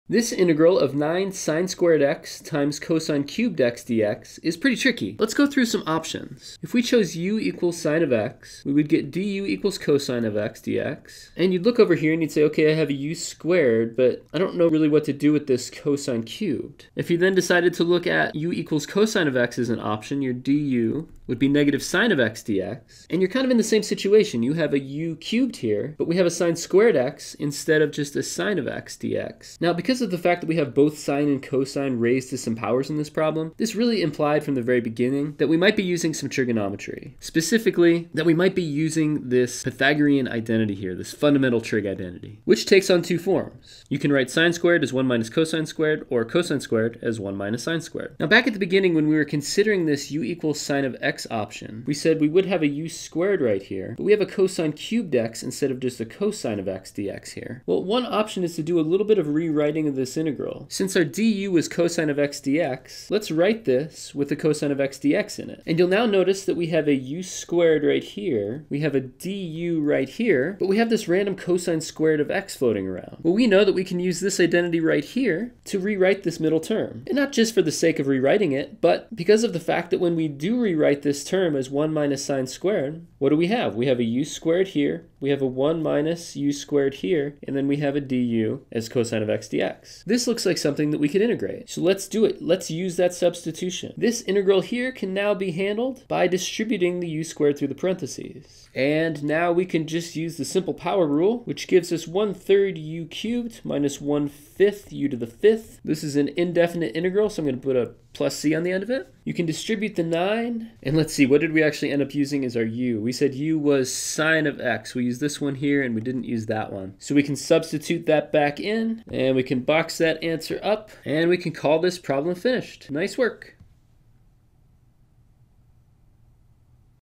Find the integral of 9 sin^(x) cos^3(x) dx
Here, we have to use my favorite trig identity to rewrite the integrand. We also need to use a u-substitution (my favorite integration technique), so we get to have twice the fun!